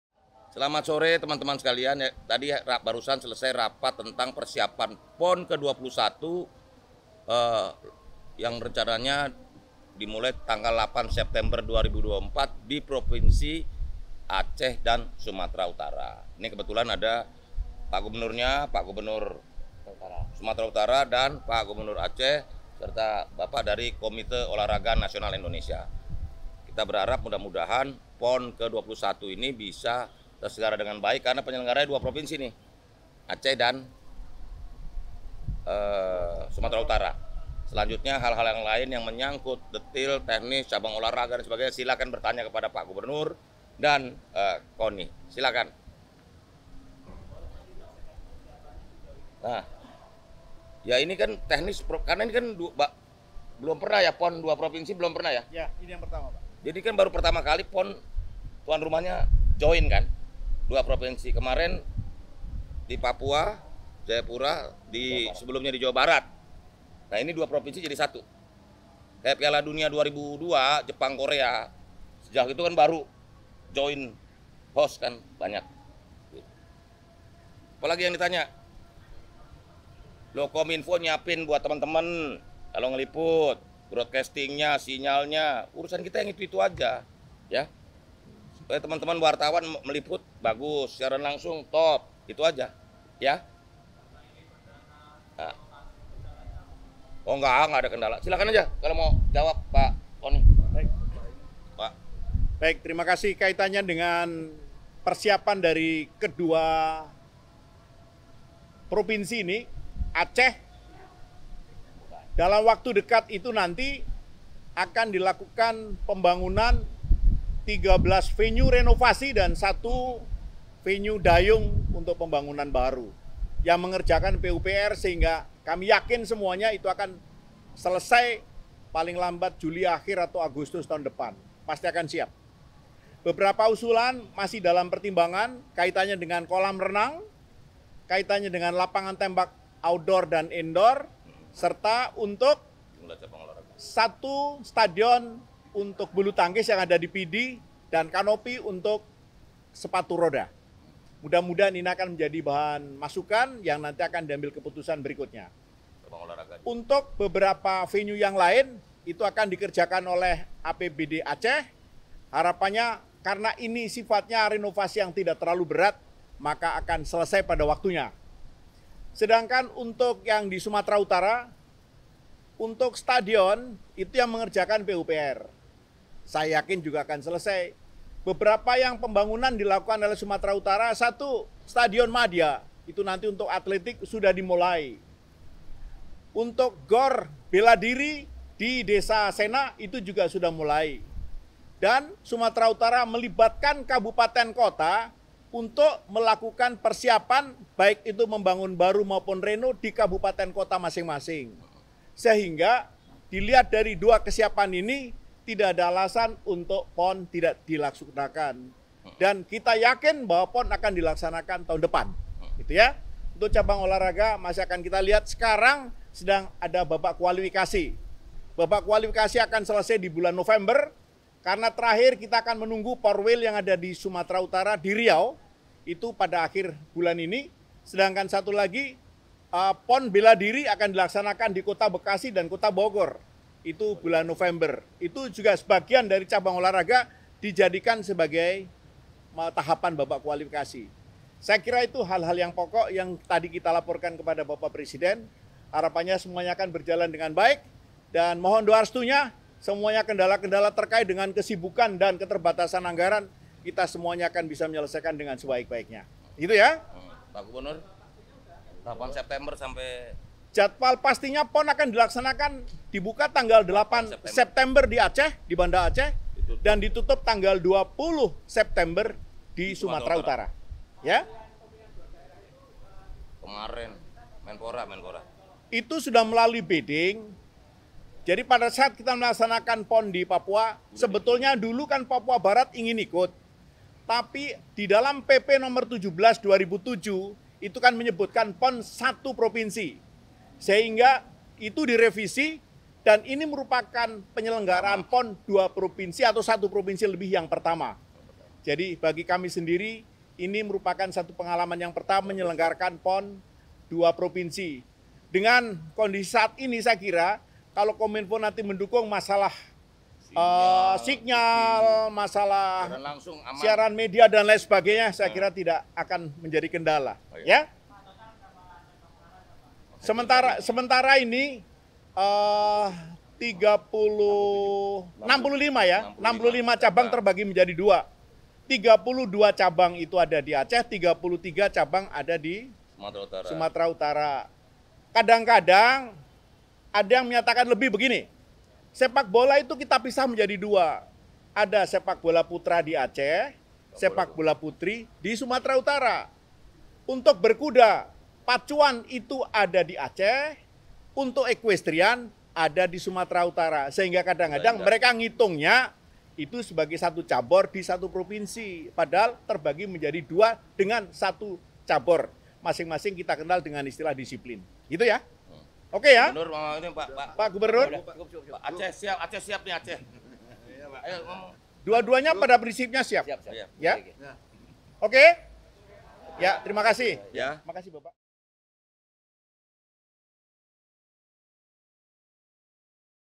Presiden Jokowi Pimpin Ratas Bahas PON XXI di Aceh-Sumut
Presiden Joko Widodo memimpin rapat terbatas (ratas) bersama jajarannya untuk membahas persiapan Pekan Olahraga Nasional (PON) XXI di Provinsi Aceh dan Sumatra Utara (Sumut) yang akan dimulai pada September 2024 mendatang. Ratas tersebut digelar di Istana Merdeka, Jakarta, pada Senin, 9 Oktober 2023.

Dalam keterangannya selepas ratas, Menteri Komunikasi dan Informatika Budi Arie Setiadi menjelaskan bahwa penyelenggaraan PON XII cukup unik karena untuk pertama kalinya ajang olahraga tersebut digelar di dua provinsi sekaligus. Ia berharap, penyelenggaraan PON tersebut bisa berjalan dengan baik.

"Kita berharap mudah-mudahan PON ke-21 ini bisa terselenggara dengan baik karena penyelenggaranya dua provinsi, Aceh dan Sumatra Utara," ujarnya di Kompleks Istana Kepresidenan, Jakarta.

Dalam kesempatan yang sama, Wakil Ketua Umum Komite Olahraga Nasional Indonesia (KONI), Suwarno, menjelaskan bahwa pemerintah terus mempersiapkan fasilitas tempat untuk penyelenggaraan PON XXI. Di Aceh, lanjutnya, dalam waktu dekat akan dilakukan renovasi 13 _venue_ dan pembangunan baru 1 _venue_ dayung.

"Yang mengerjakan PUPR sehingga kami yakin semuanya itu akan selesai paling lambat Juli akhir atau Agustus tahun depan pasti akan siap," ungkapnya.

Selain itu, usulan fasilitas lain yang masih dalam pertimbangan antara lain kolam renang, lapangan tembak dalam dan luar ruangan, stadion bulu tangkis, dan arena sepatu roda. Menurutnya, pembangunan tersebut akan dikerjakan dengan anggaran pendapatan dan belanja daerah (APBD) Aceh.

"Harapannya karena ini sifatnya renovasi yang tidak terlalu berat maka akan selesai pada waktunya," imbuhnya.

Suwarno meyakini pembangunan fasilitas olahraga di Sumatra Utara juga akan selesai pada waktunya. Beberapa pembangunan yang dilakukan antara lain stadion madya untuk atletik, gelanggang olahraga untuk bela diri.

Ia melanjutkan, pembangunan maupun renovasi fasilitas olahraga juga melibatkan pemerintah kabupaten/kota masing-masing. Ia pun yakin PON XXI akan digelar sesuai waktu yang dijadwalkan pada tahun depan.

"Dilihat dari dua kesiapan ini tidak ada alasan untuk PON tidak dilaksanakan dan kita yakin bahwa PON akan dilaksanakan tahun depan," ucapnya.

Untuk diketahui, PON XXI rencananya akan dibuka pada 8 September 2024 di Aceh dan ditutup pada 20 September 2024 di Sumatra Utara.